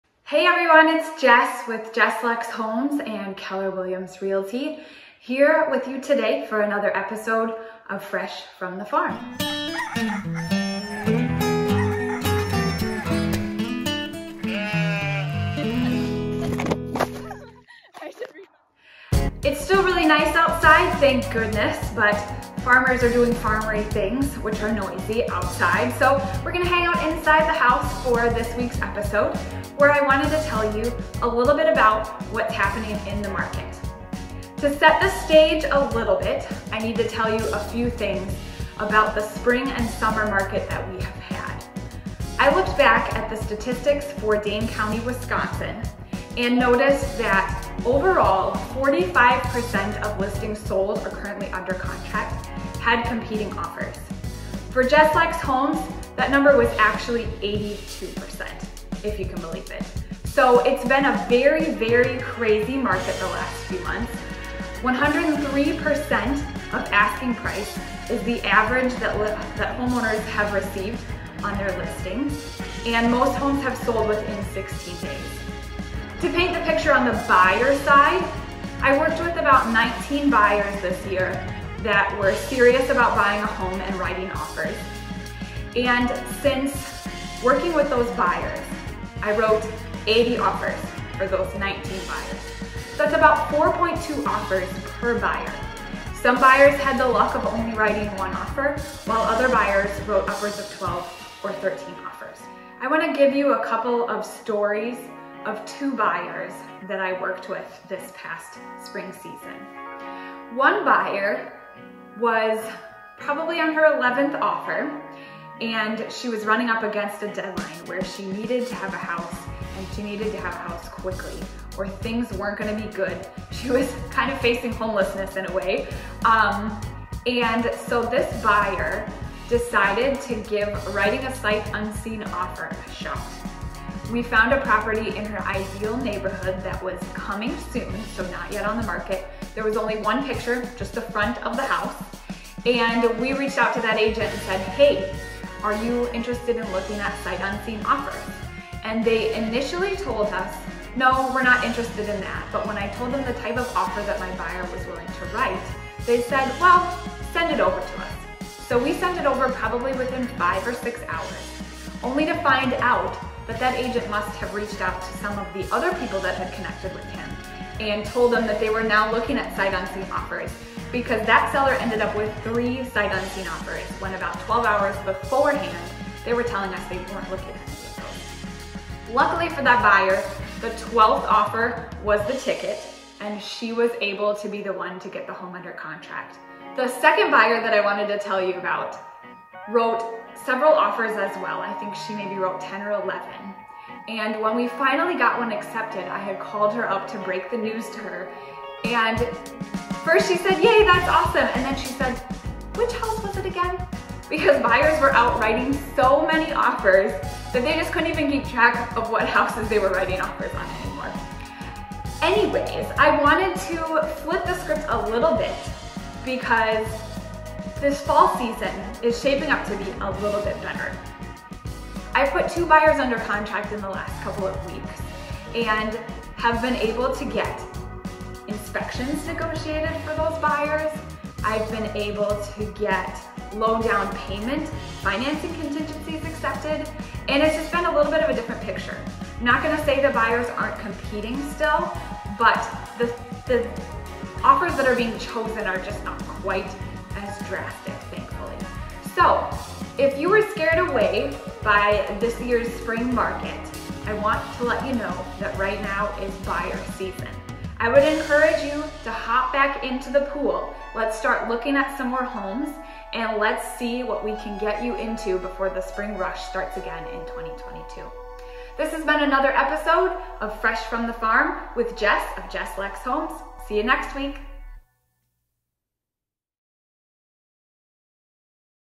Market Update: Time for Buyers to Jump Back In - Fresh from the Farm Ep. 9
This week Jess gives you a snapshot of how crazy the market was this summer with a couple buyer anecdotes. But never fear... there is hope on the horizon.
Want to hear more about why this season is a great time for buyers? Keep an eye out for our newsletter this November. We'll give you some reasons why winter can be the most advantageous time to buy!